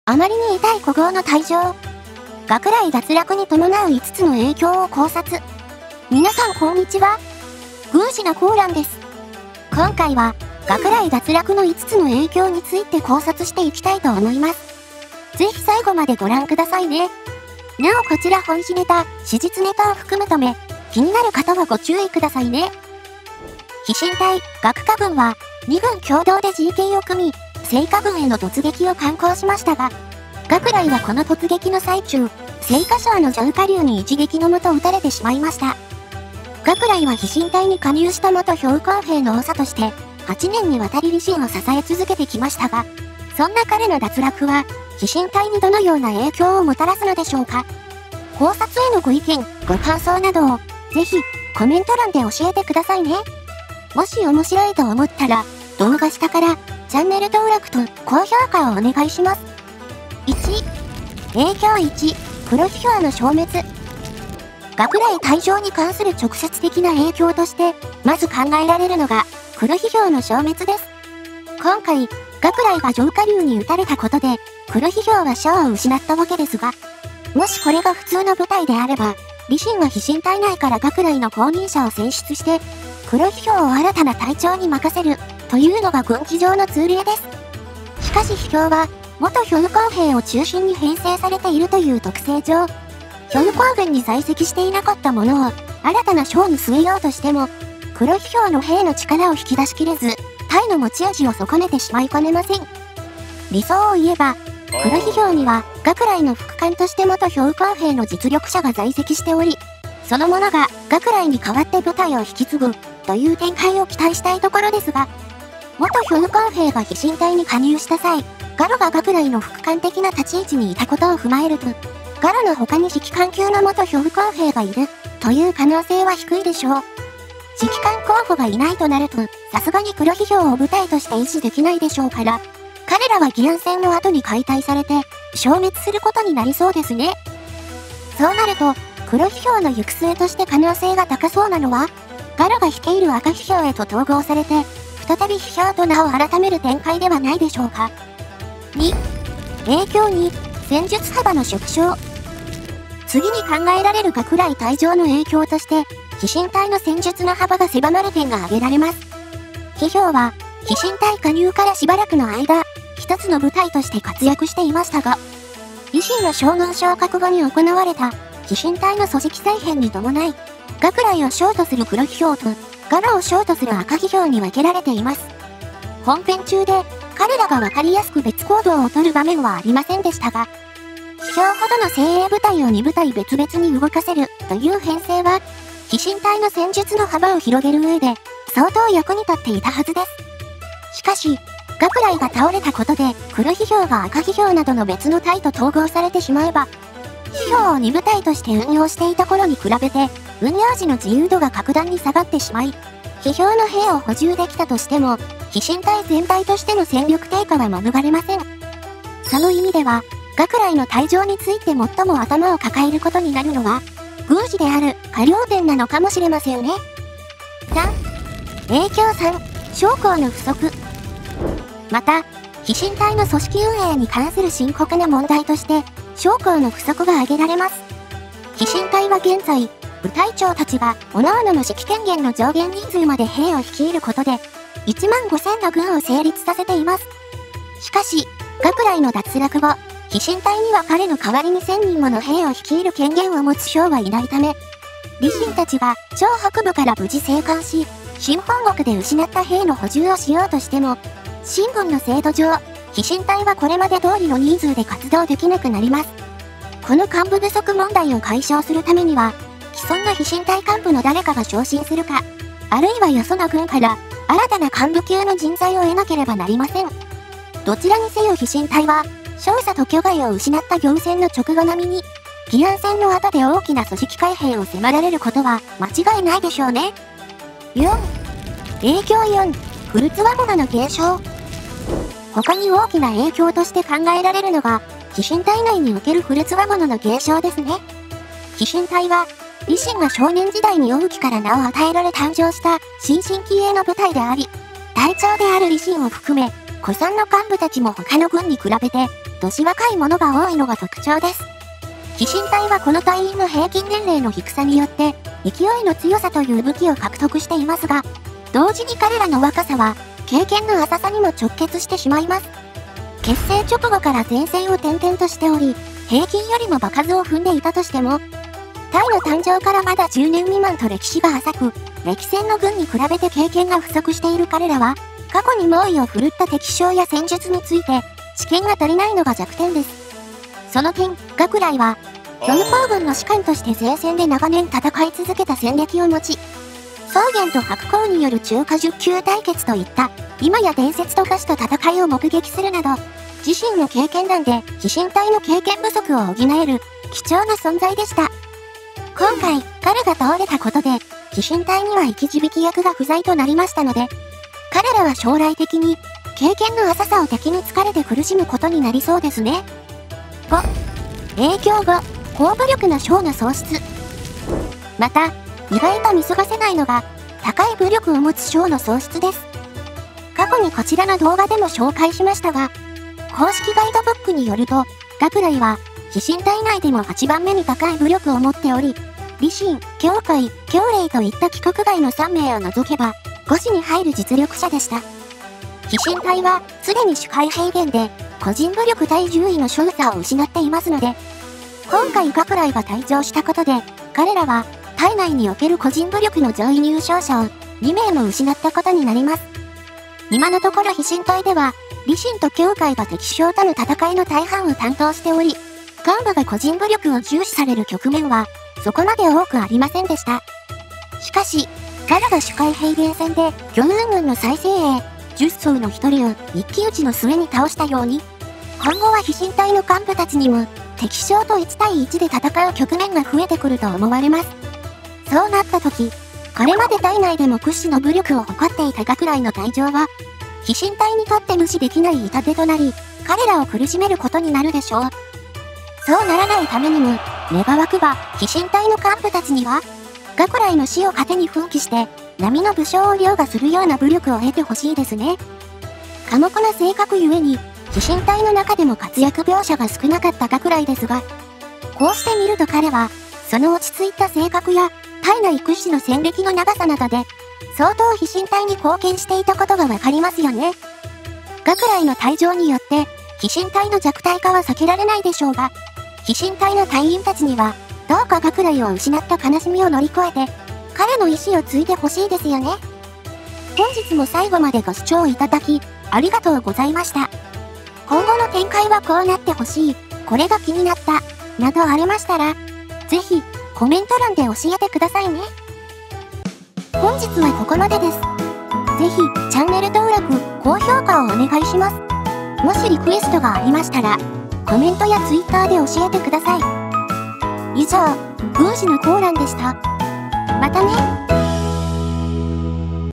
【キングダム】弱体化は不可避!?岳雷脱落による飛信隊への影響を考察【キングダム考察】
キングダムで飛信隊を長らく支えてきた岳雷は青歌軍の上和龍に討たれてしまいましたが
岳雷が抜けることによって、飛信隊には５つもの大きな影響が現れそうです…。
今回はそんな岳雷退場による飛信隊への影響について考察してきます。

キングダムの考察や史実の研究についての動画を配信しています！
あとベルマークを押して通知をオンにしてもらえると嬉しいです。